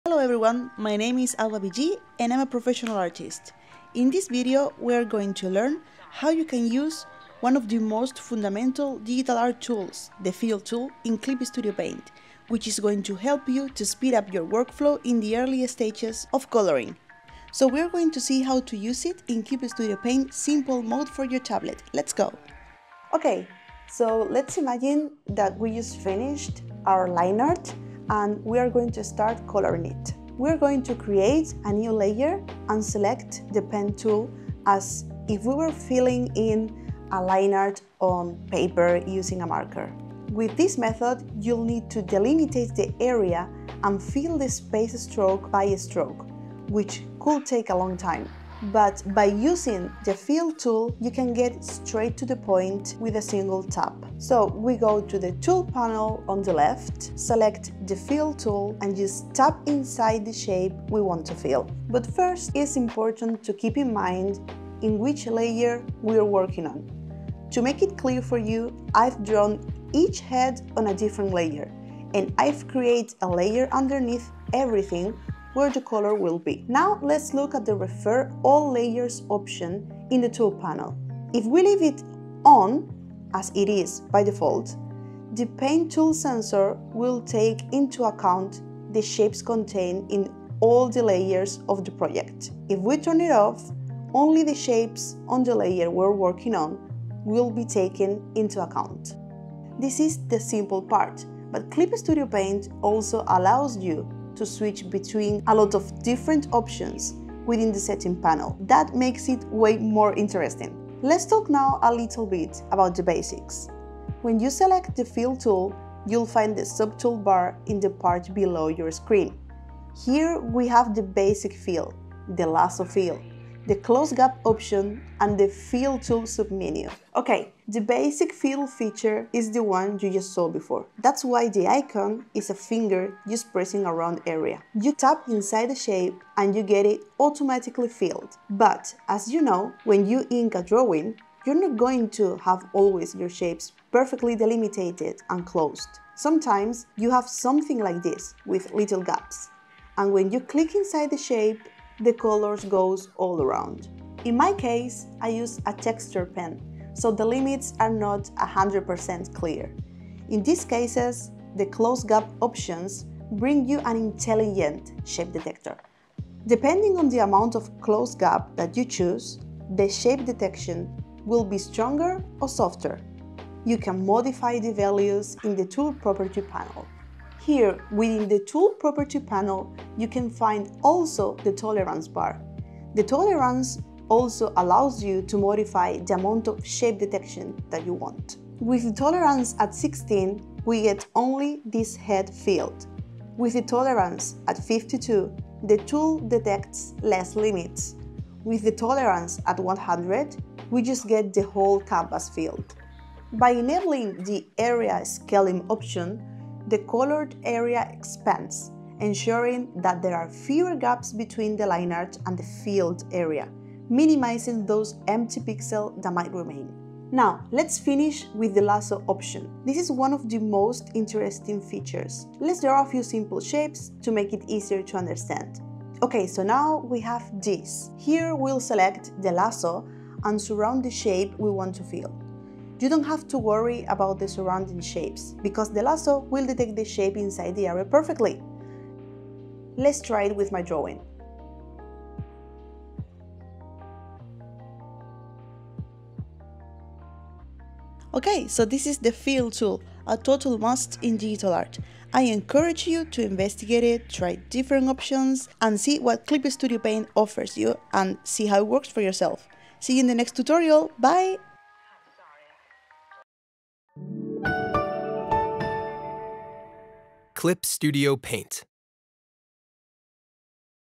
Master the Fill tool in Simple Mode with AlbaBG | Clip Studio Paint Recommended Feature
Learn all about the Fill tool in Clip Studio Paint Simple Mode.
AlbaBG offers an insightful tutorial on the various fill methods available in Simple Mode, ranging from the basic Fill tool to the Lasso tool.
This tutorial also covers how to use features like Refer all layers, Close Gap, Tolerance, and Area scaling that can help you color your drawings accurately and with ease.
Follow along and see which Fill tool works for your drawing style.

0:00 Introduction to the Fill tool
1:17 The Refer all layers option
1:51 The different Fill options
2:19 Fill accurately with Close gap
2:57 Tolerance and shape detection
3:23 More coverage with Area scaling
3:37 Lasso Fill demonstration

Find out more about the Fill Tool below.